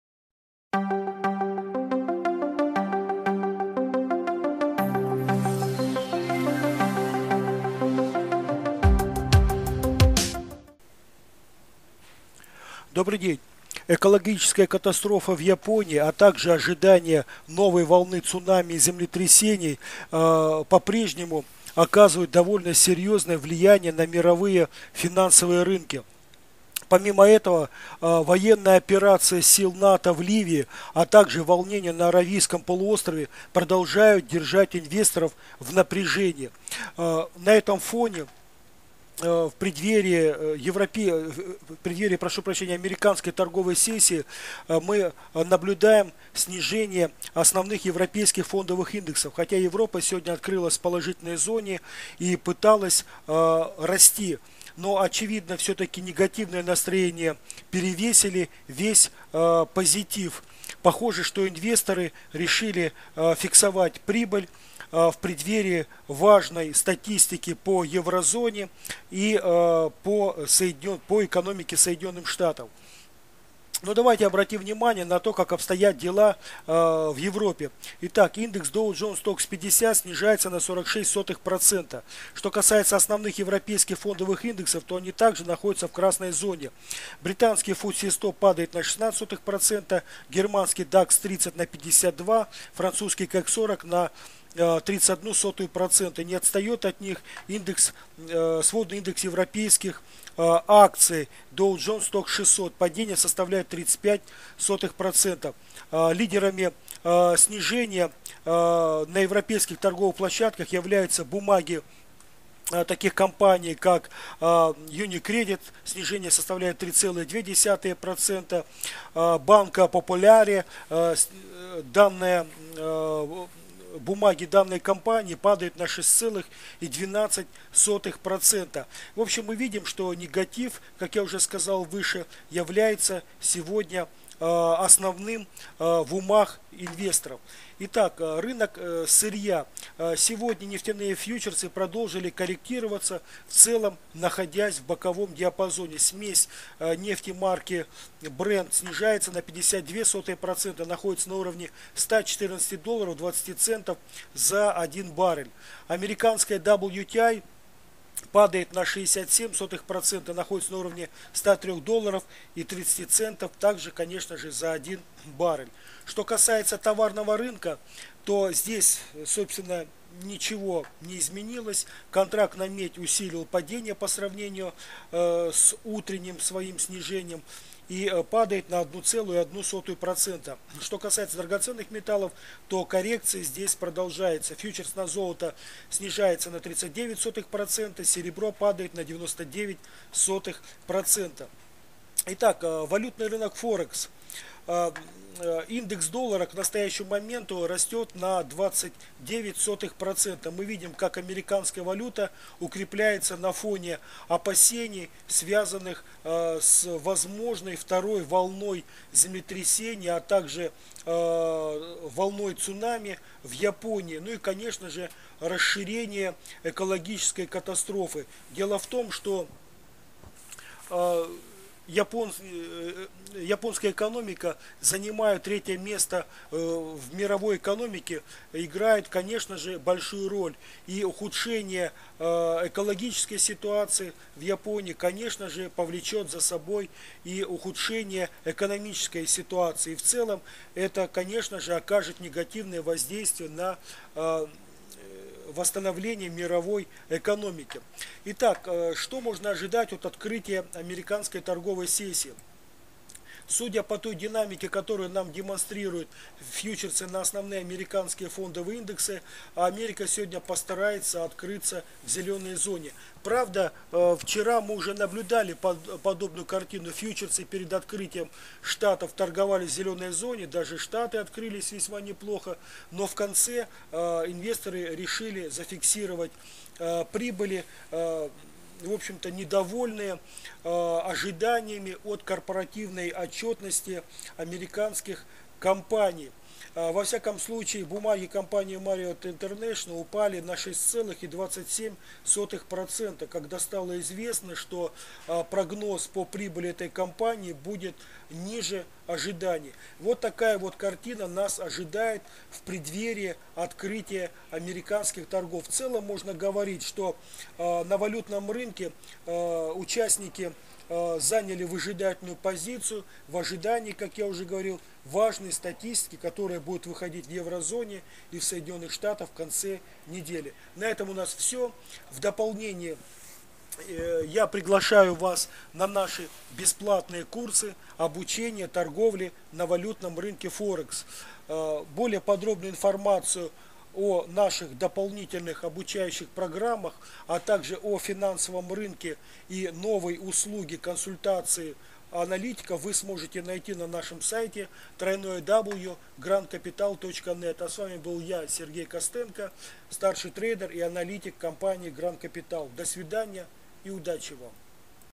29 03 2011 г Обзор к открытию американской торговой сессии
Важные события дня, основные тенденции на мировом финансовом рынке, торговые рекомендации.